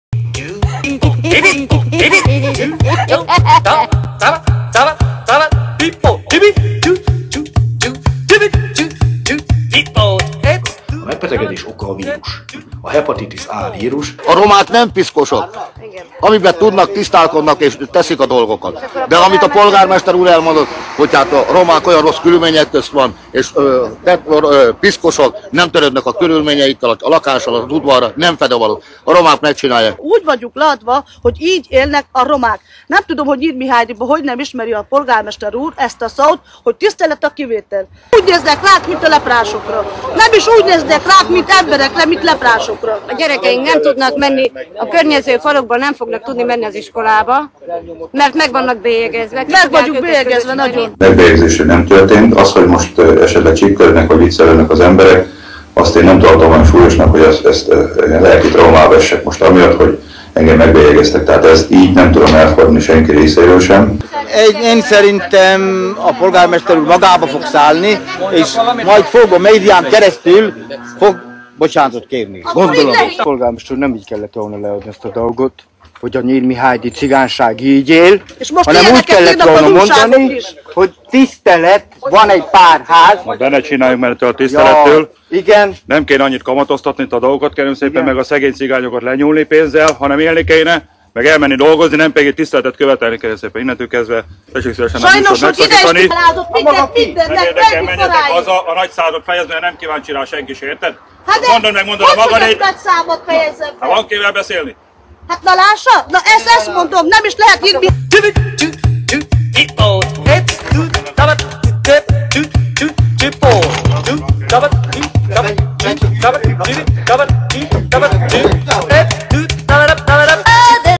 Hepatitis járvány - a cigányok még vécét sem használtak
Néhány napja Hepatitis A járvány tört ki Nyírmihálydiban. A vírus széklettel terjed, a középkori szinten élő cigányok viszont nem használnak vécét, így megteremtették az ideális állapotot a betegség számára. Nyírmihálydi polgármestere nagyon helyesen kijelentette:  az egyébként kizárólag segélyekből élő cigányok  addig nem kapnak segélyt, amíg nem  építenek budit - a XXI. században talán ez már elvárható.  A cigók rasszizmusról beszélnek, meg hogy nekik milyen rossz: leprásnak nevezik őket (ezek után joggal), meg vannak bélyegezve stb. Az Index videóján jól látható: a polgármester kijelentette: nem kér a nyavajgásból, a cigányok elmehetnek dolgozni...